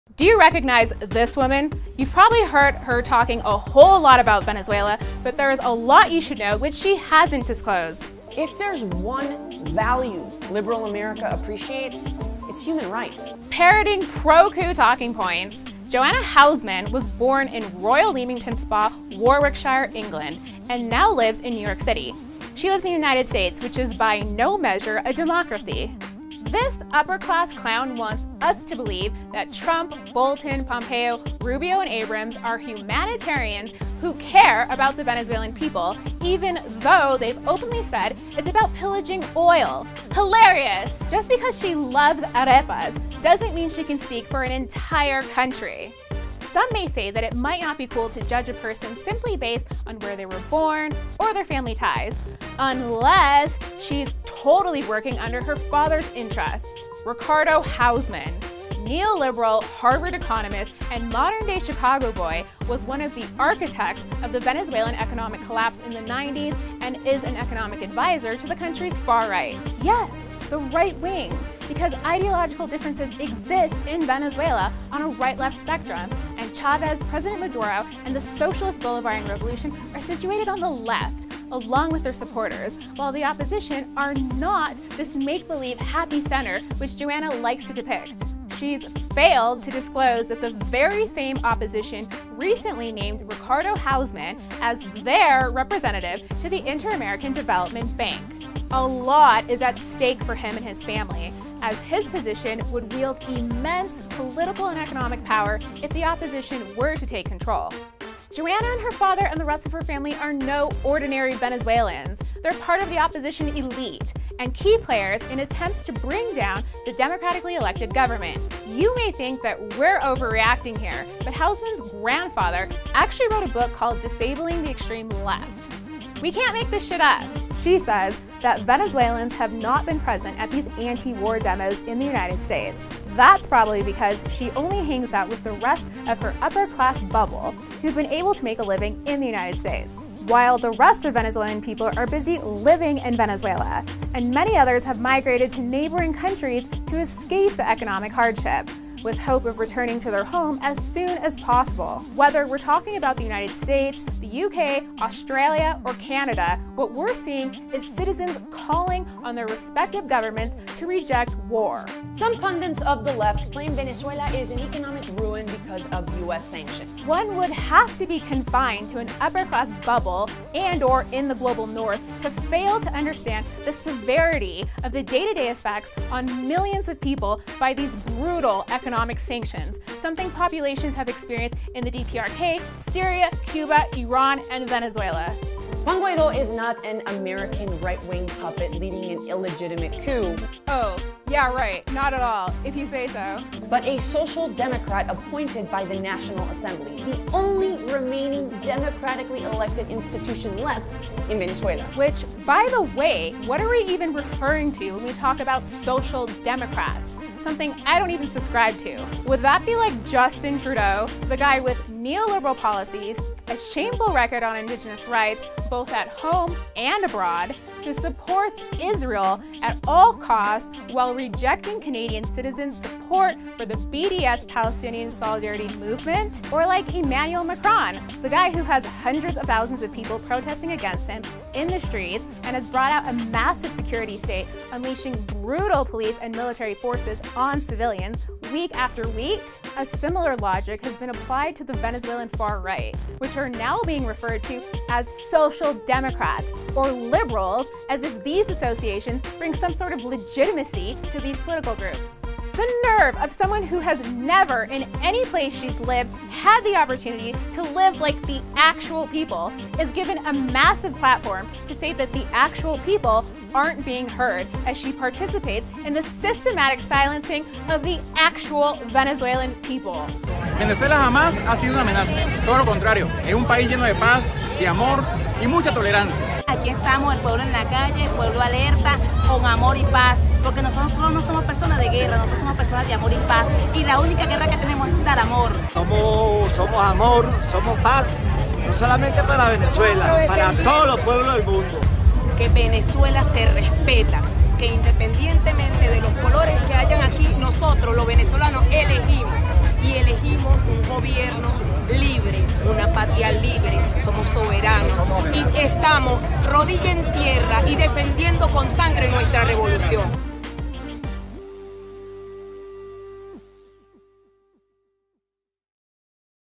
Pro-Coup Youtuber
Joanna Hausmann: Making Trump’s coup more palatable for millennials.

But there’s plenty she hasn’t disclosed.